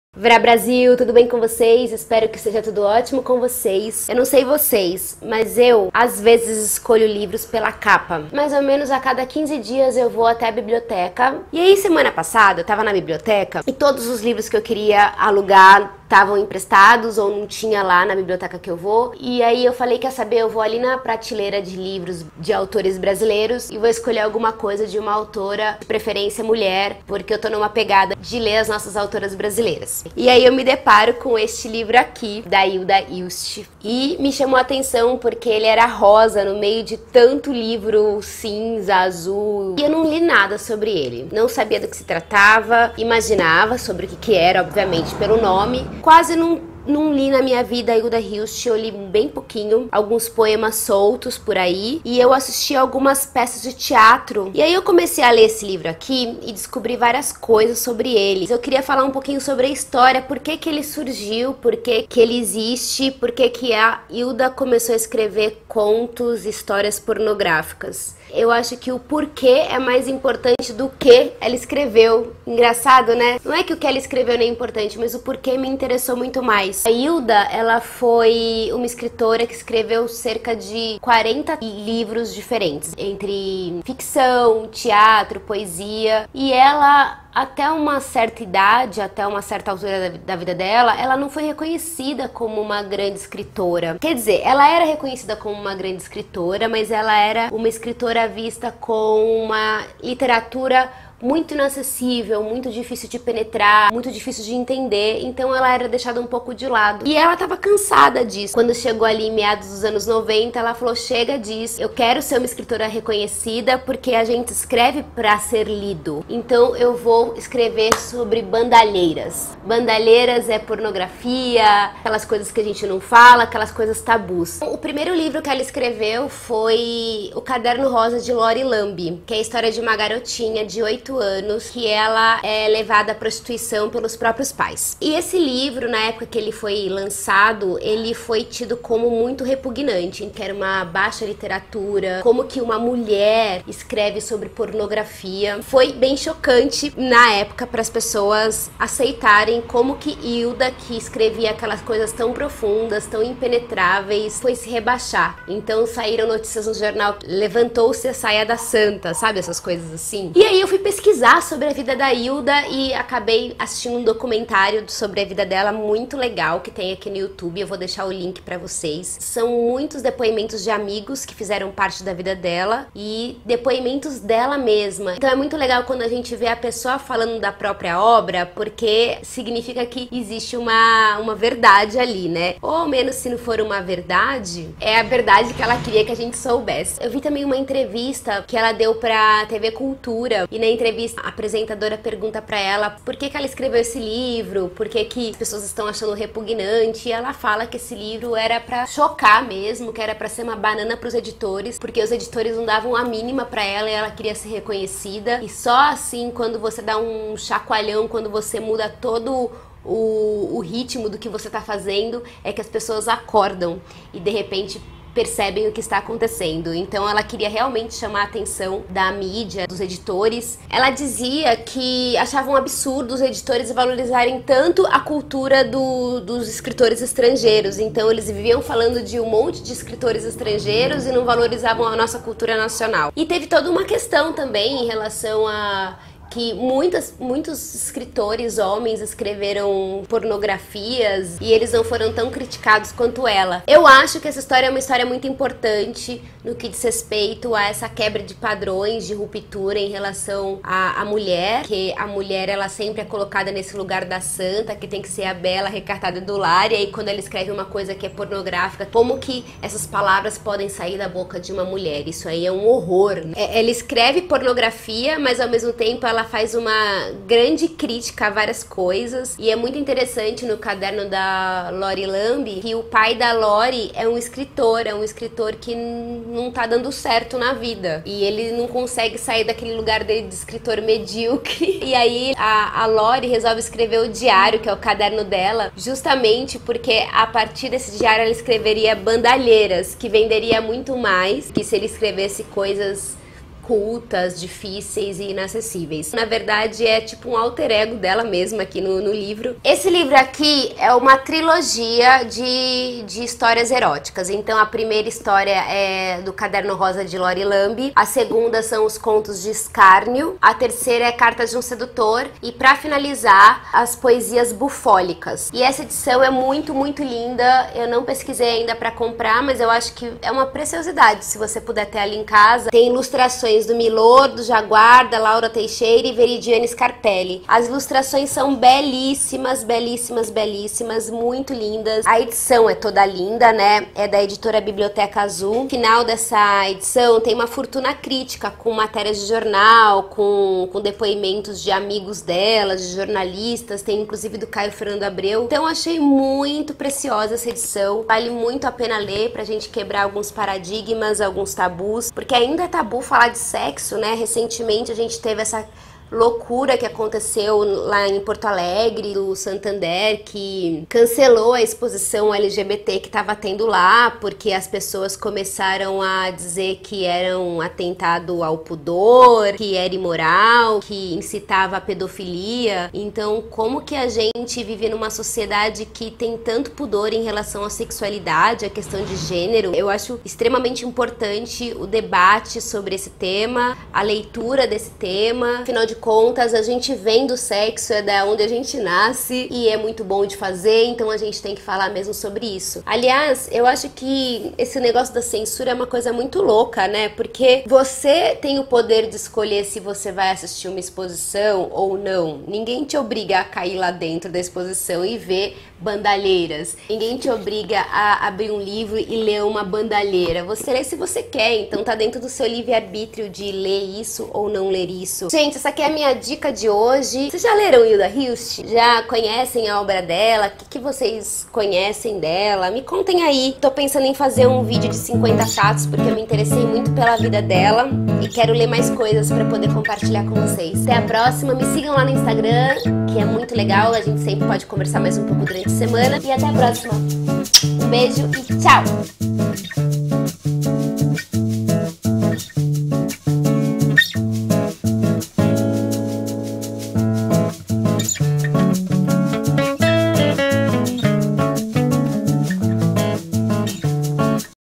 POR QUE HILDA HILST ESCREVEU PORNOGRAFIAS
Resenha do livro Porno Chic, de Hilda Hilst

✅ Escreva para mim:
CAIXA POSTAL 13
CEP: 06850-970